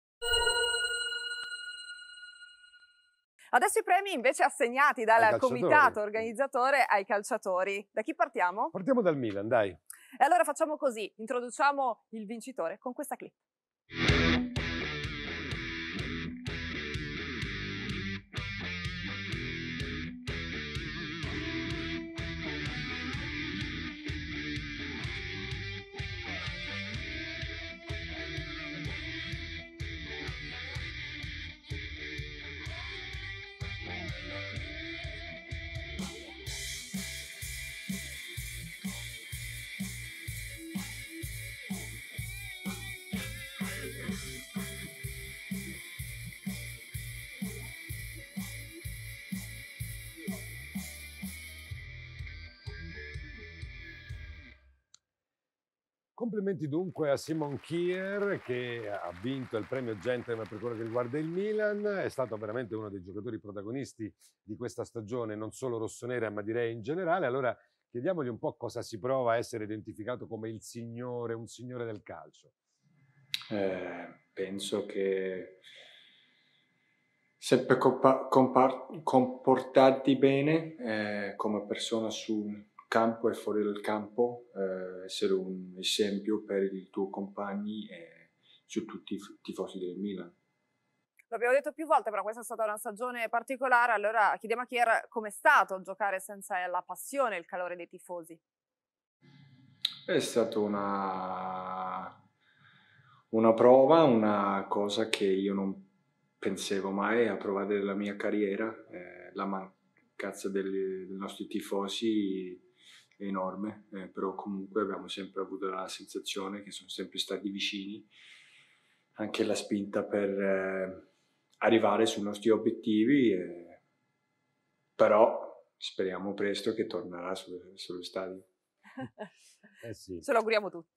Premio Gentlemen Digital Edition 2021 Milan Kjaer
Il Premio Gentleman 2021 A.C. Milan viene assegnato a Simon Kjaer